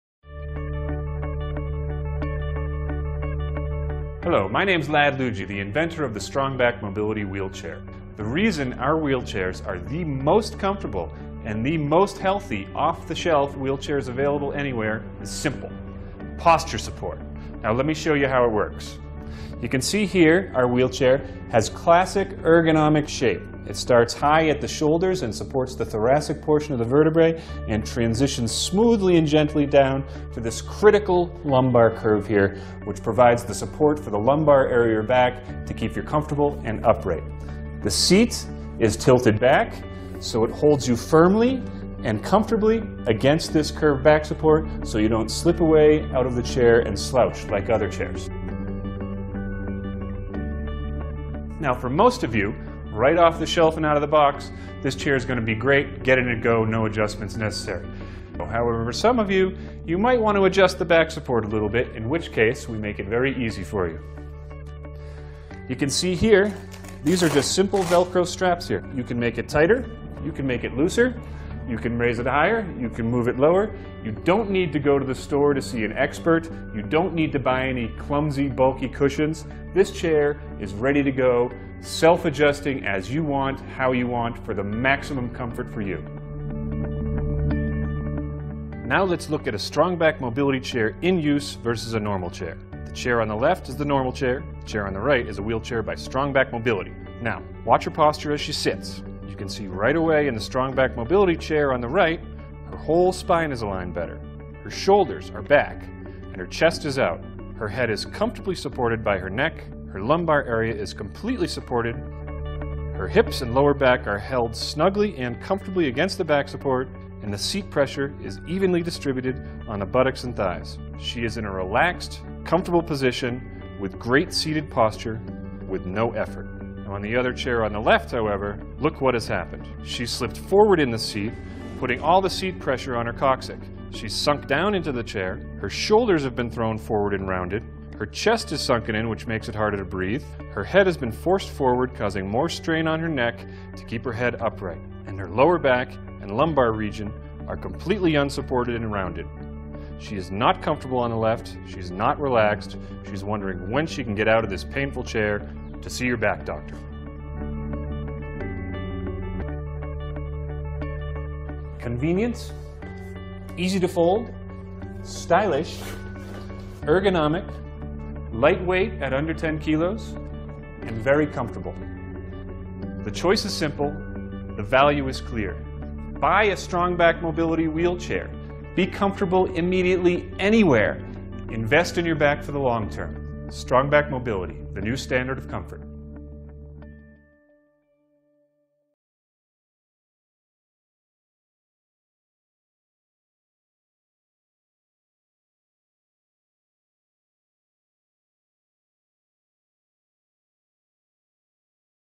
Strongback Mobility Wheelchairs Explained iim
Unlike standard folding manual wheelchairs, the STRONGBACK wheelchair by Strongback Mobility conforms to and supports the natural and healthy lumbar curve of the spine thus preventing the user from slouching forward and squeezing spinal discs with a tremendous amount of unhealthy pressure - decreasing this pressure results in healthier discs, a stronger back, and long-lasting comfort while seated.